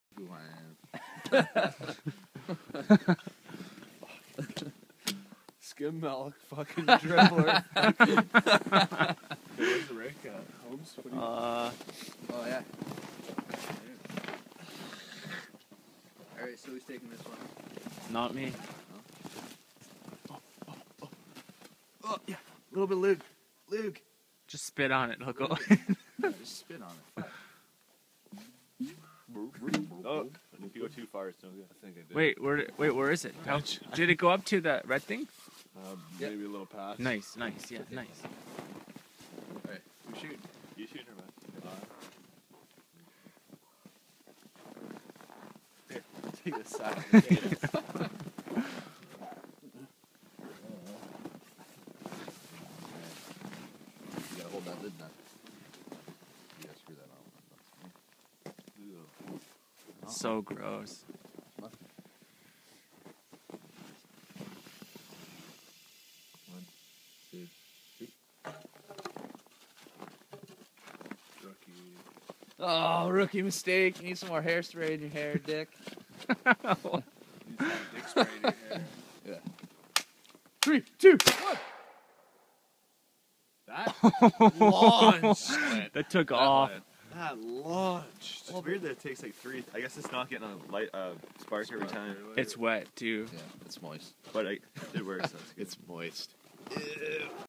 The Real Jersey Shore - Heartbreak at Balsam (Shooting the Potato Cannon)
This would do some serious damage... Balsam Lake Ontario Canada